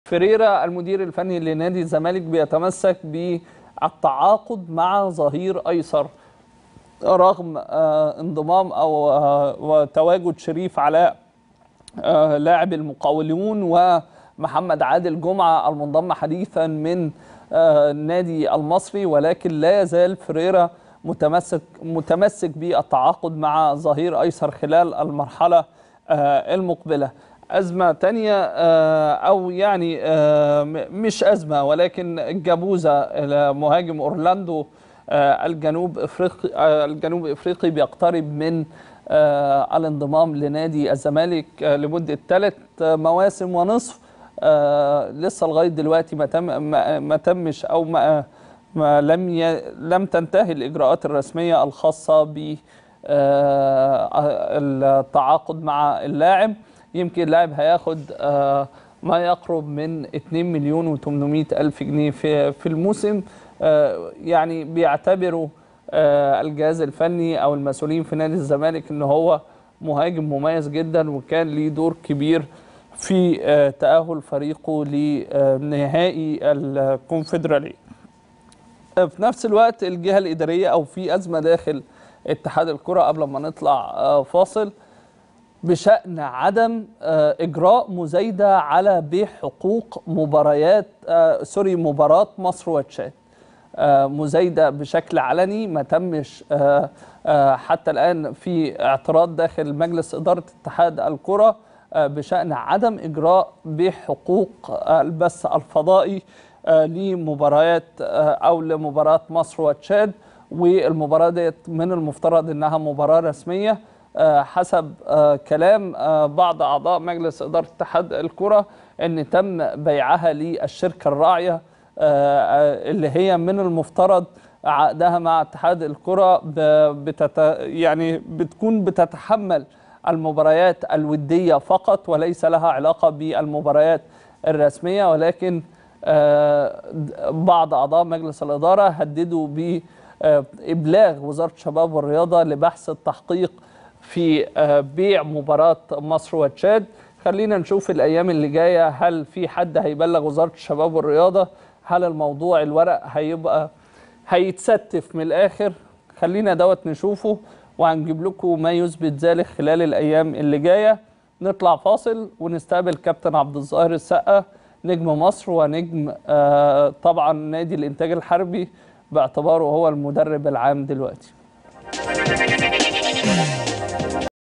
بالورقة والقلم | فيريرا يطلب التعاقد مع ظهير ايسر واقتراب مهاجم اورلاندو من الزمالك
فيريرا يطلب التعاقد مع ظهير ايسر واقتراب مهاجم اورلاندو يقترب من الزمالك